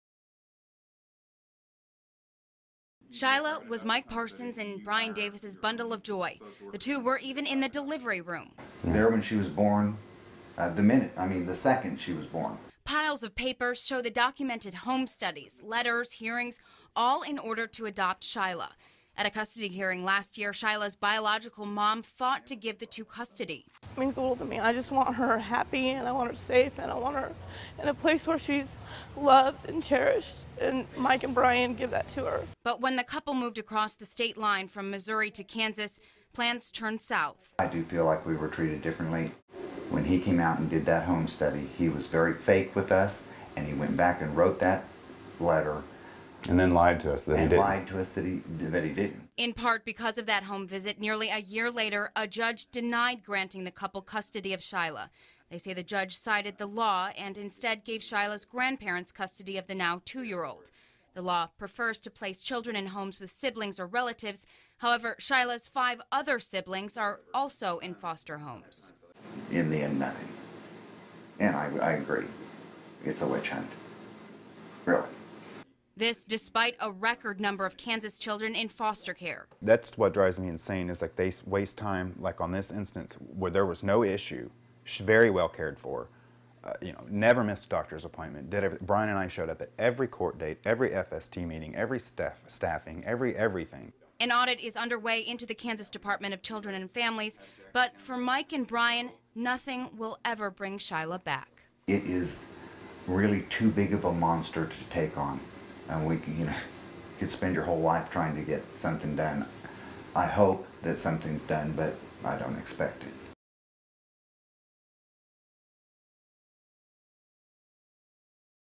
Same-sex couple says state of Kansas targeted them
An Overland Park same-sex couple fought to regain custody of the child they raised and planned to adopt, but in the end, they lost.

41 Action News, KSHB, brings you the latest news, weather and investigative reports from both sides of the state line.